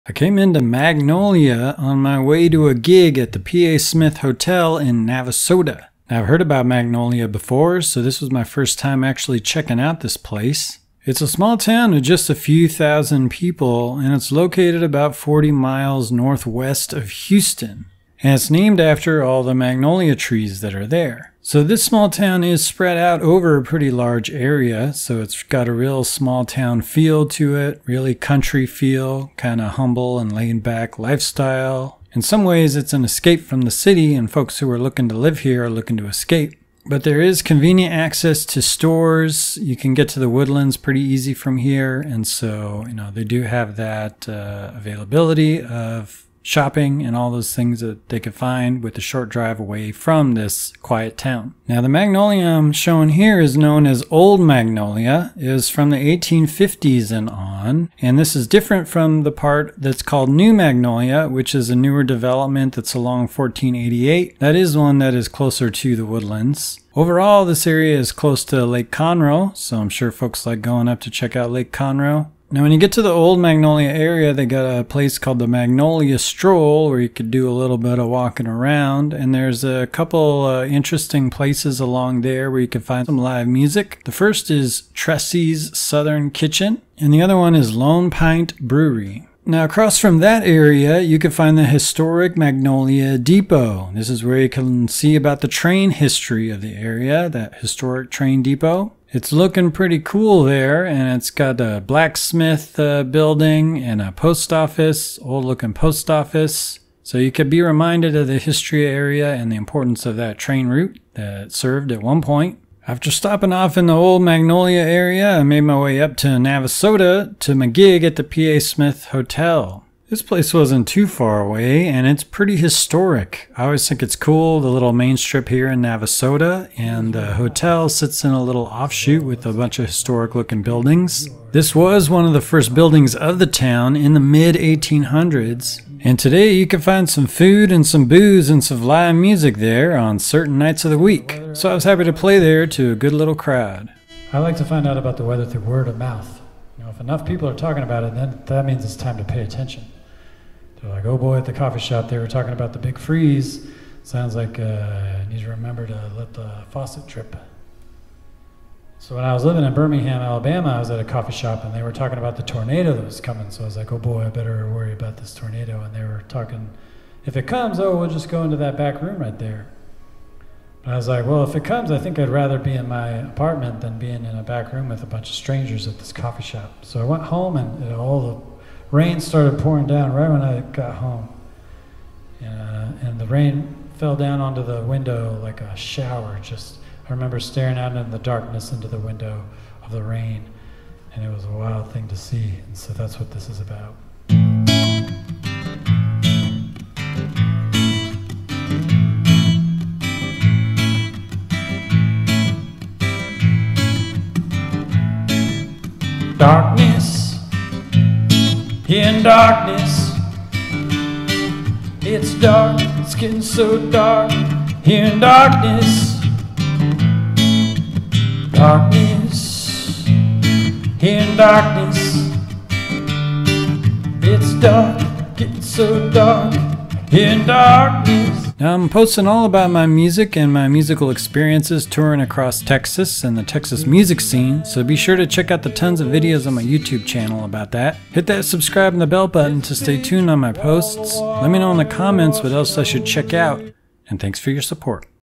A tour of Magnolia, Texas (A neighborhood outside of Houston)
Magnolia is a small town of a few thousand people, located 40 miles NW of Houston. The area that originated around the 1850's is known as Old Magnolia and the more recent development to the east of that is known as New Magnolia. Each areas have pretty easy access to the Woodlands for shopping and more entertainment as well as to Lake Conroe for lake activities. You can find live music at Tressie's Southern Kitchen and Lone Pint Brewery. After checking out Old Magnolia I went to my gig at the P.A. Smith Hotel in Navasota. Good times.